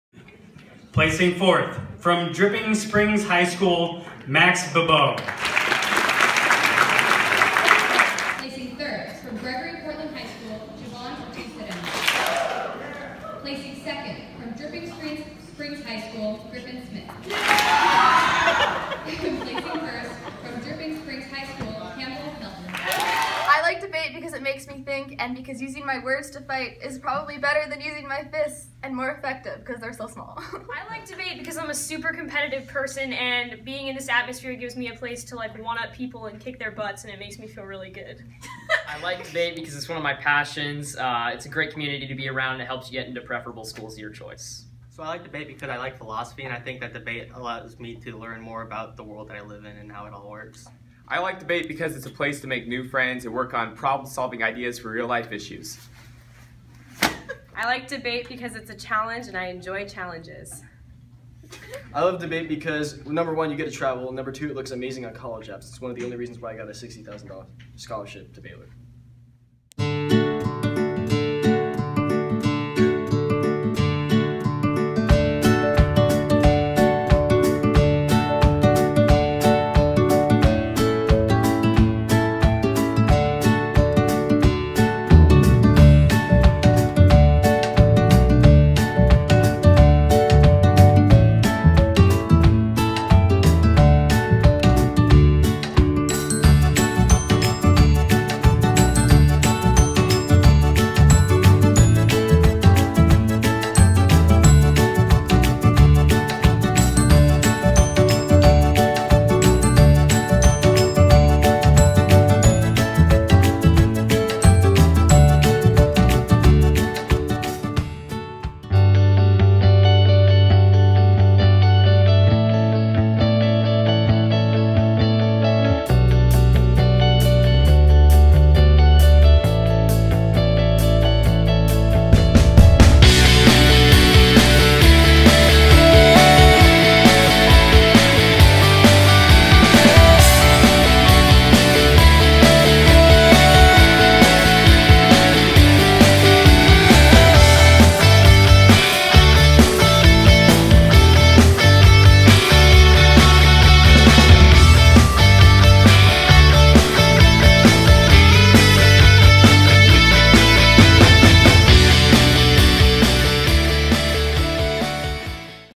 Debate I-Course Introduction
A description of the Debate I course at Dripping Springs High School and a promotional video for the Dripping Springs High School Speech and Debate Team.